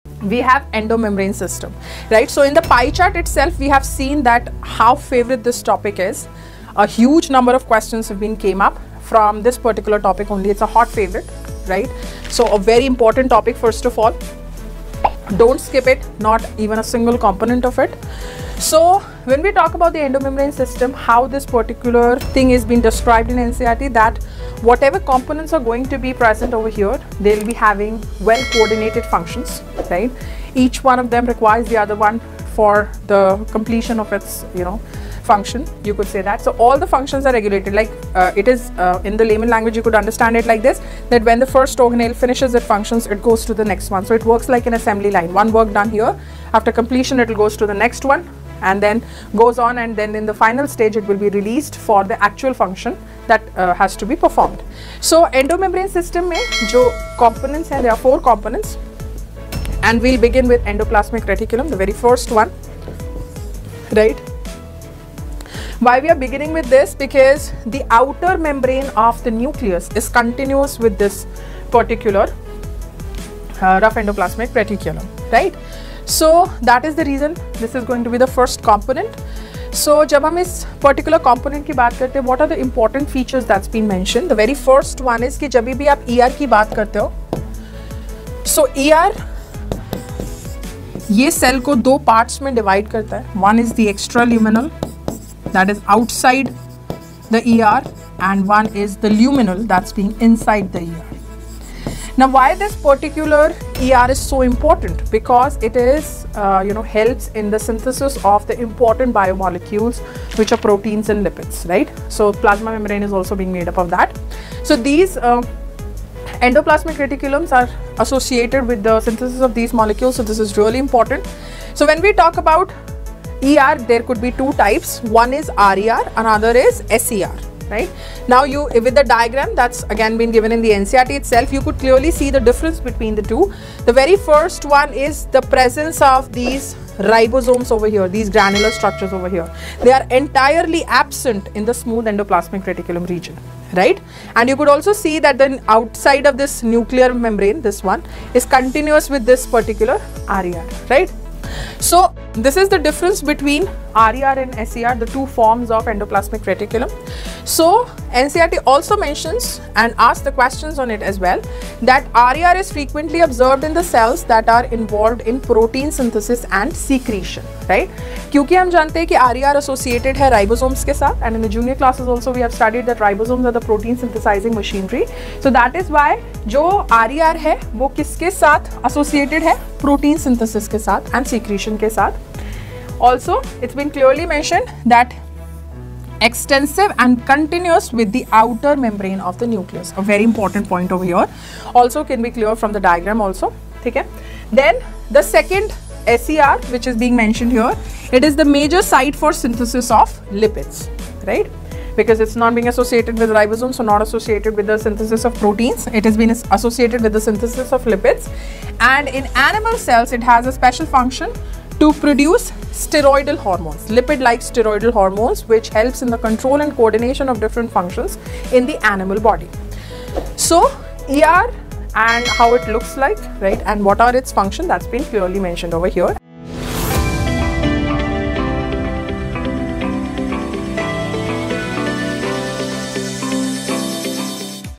ER Made Super Easy 🔍 Students Wish They Learned This Earlier | NEET 2026 ✨
Confused about the Endoplasmic Reticulum (ER) for NEET 2026? This video simplifies the entire concept—RER vs SER, structure, functions, protein synthesis, lipid metabolism, NCERT points, and exam-focused tricks.

Perfect for NEET aspirants who want crystal-clear understanding in the shortest time.

Never miss an update:
✅For admission to Aakash, register for iACST - your gateway to up to 90% scholarship